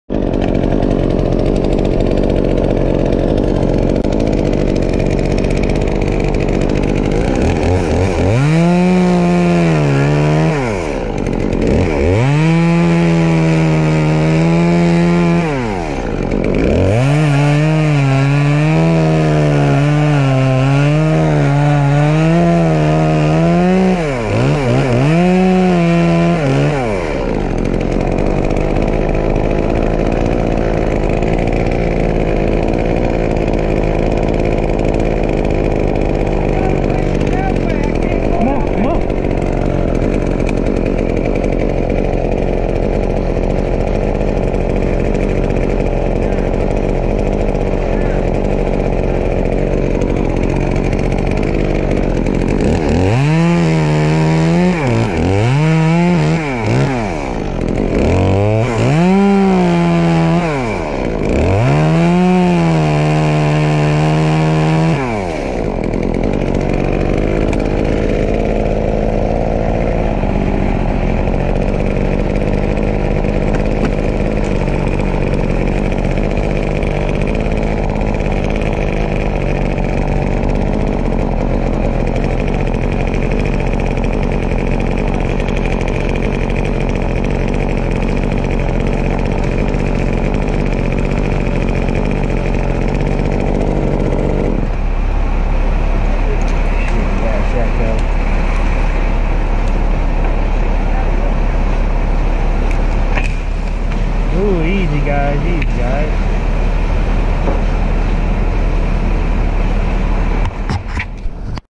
Limb removal with knuckle boom crane on Burgett Lane in Canfield, Ohio 10/5/2018
Doing my first pick with a 35 foot knuckle boom crane to remove a limb on this locust tree that was overhanging some utility lines. I could have rigged it out with rope and pulley along with the portawrap but we just made it a little bit easier using the crane. The most irritating thing that happened to me is that I ended up dropping my Stihl 201TC just moments before this pick and I ended up having to use the most shittiest saw you can use AKA an Echo 355T.

Braun's Tree Service and Stump Removal